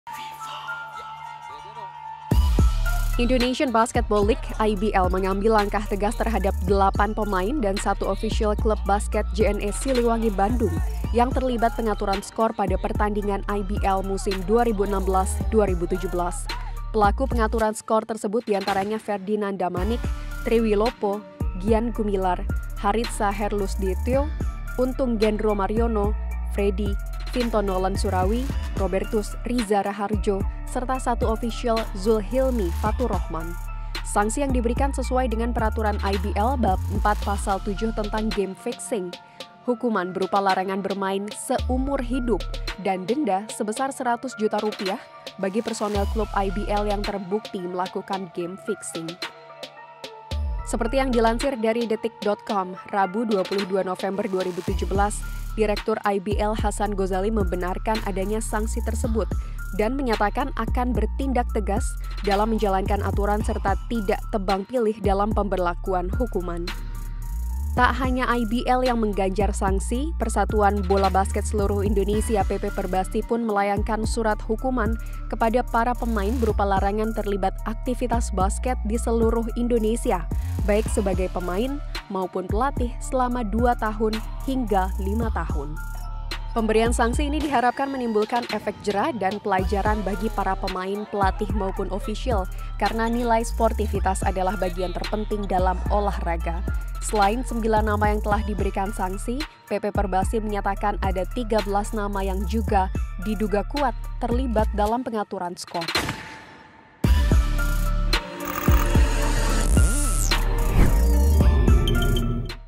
Terlibat Pengaturan Skor, 8 Pemain IBL dilarang Bermain Seumur Hidup
Download aplikasi VisualTV.Live:

Akses dan Follow Official Account VISUALTV.LIVE di:

- Line: @VisualTVdotLive
- Path: @visualtvlive